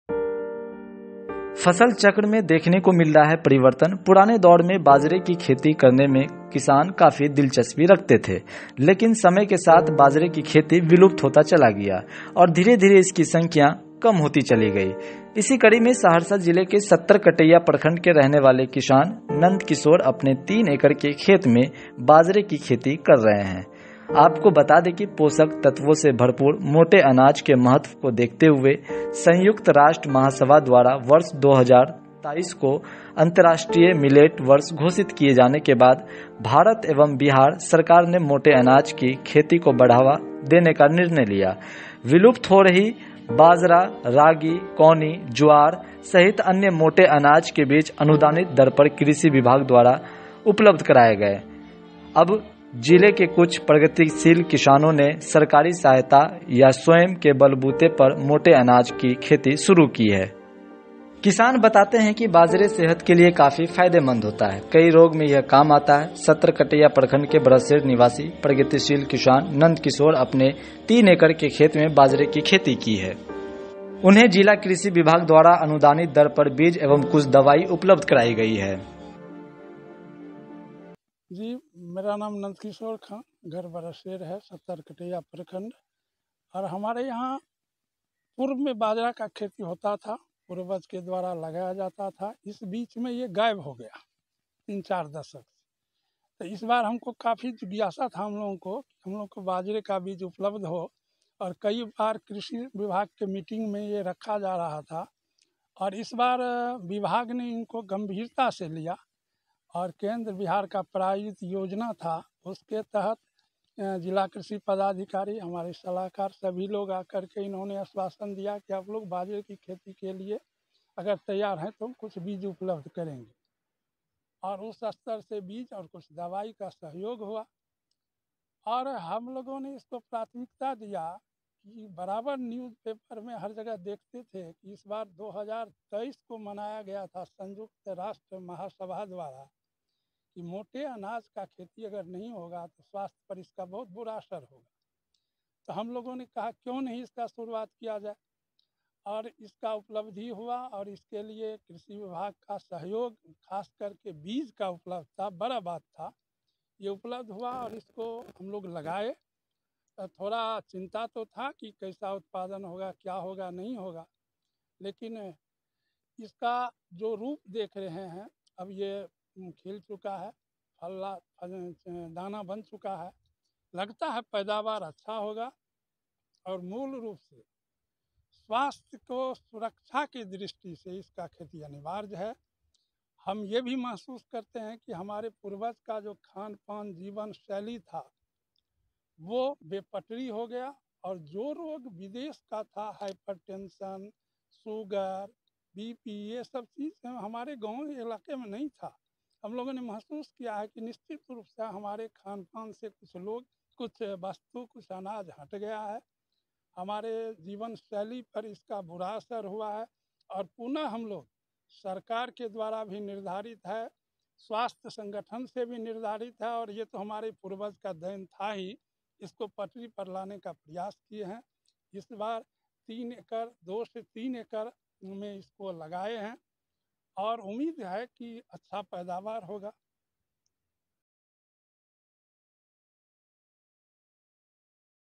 Millets Farming: धान की जगह बाजरे की खेती करेगी किसानों को अमीर! अपनाएं ये Farming Tips
Nandkishore, a farmer of Sattar Kataiya block of Saharsa district, has started farming of millet in 3 acres. After the United Nations declared 2024 as the International Year of Millets, the Government of India and Bihar decided to promote the cultivation of the millet. Seeds and medicines are being given to farmers at subsidized rates. Nandkishore says that millet is now flourishing in paddy and wheat fields, which costs half as much as paddy. There is increasing happiness and enthusiasm among the farmers regarding millet cultivation.

सहरसा जिले के सत्तर कटैया प्रखंड के किसान नंदकिशोर ने 3 एकड़ में बाजरे की खेती शुरू की है। संयुक्त राष्ट्र द्वारा 2024 को अंतरराष्ट्रीय मिलेट वर्ष घोषित करने के बाद, भारत और बिहार सरकार ने मोटे अनाज की खेती को बढ़ावा देने का निर्णय लिया। किसानों को अनुदानित दर पर बीज और दवाइयां दी जा रही हैं। नंदकिशोर बताते हैं कि धान और गेहूं के खेतों में अब बाजरा लहलहा रहा है, जिसमें धान की तुलना में आधी लागत आती है। किसानों में बाजरे की खेती को लेकर खुशी और उत्साह बढ़ रहा है।